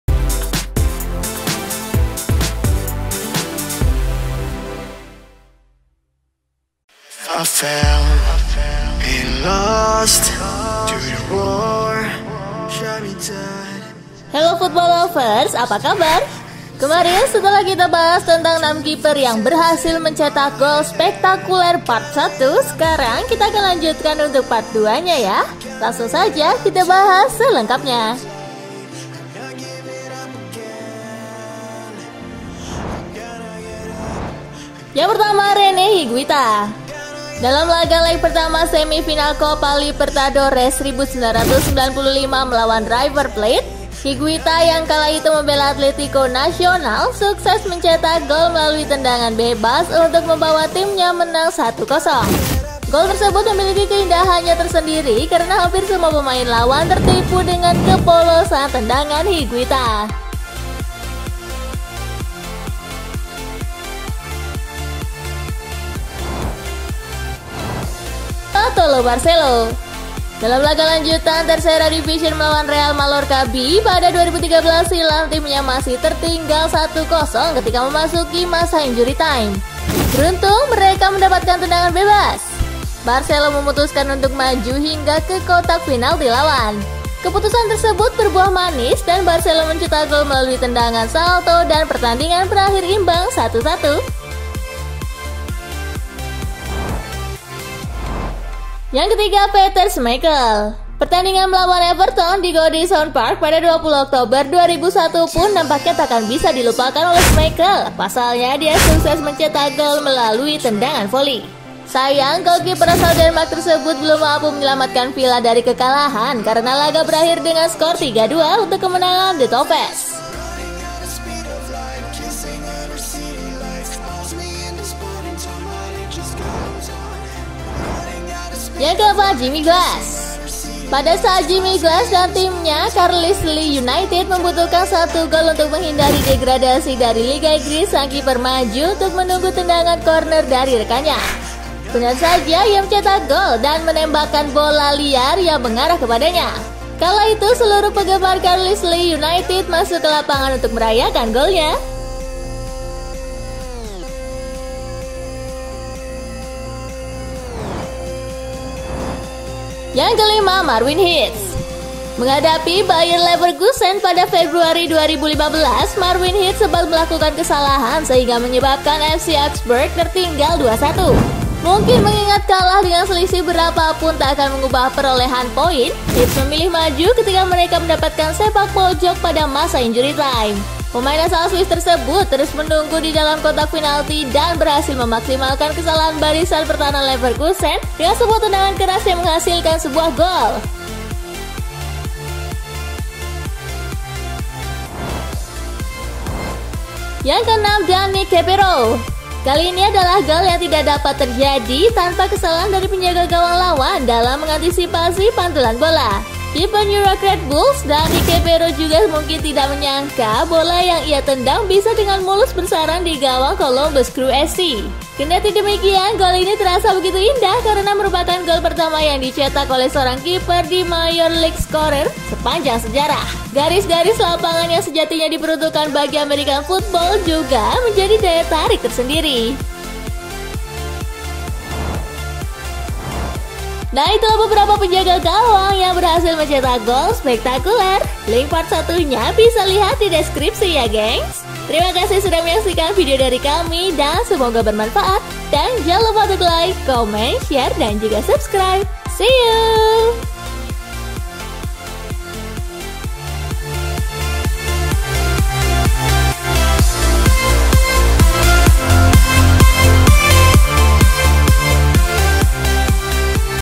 6 Penjaga Gawang Yang Mencetak Goal Spektakuler part 2
Kemarin setelah kita bahas tentang 6 kiper yang berhasil mencetak goal spektakuler part 1 , sekarang kita akan lanjutkan untuk part 2 nya ya??
Langsung saja kita bahas selangkapnya..